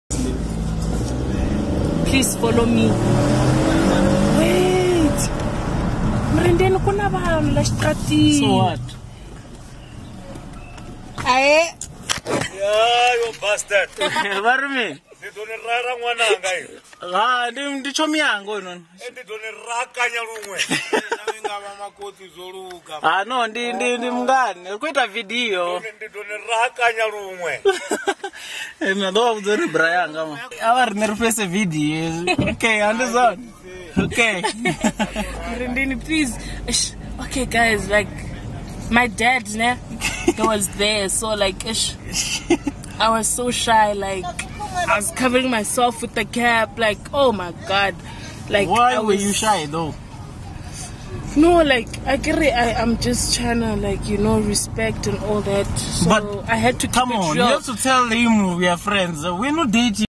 Her Dad wants to Fight Me for Speed 😂😂
I met My sister from another mother's Dad he wants to fight me he thought am... His daughter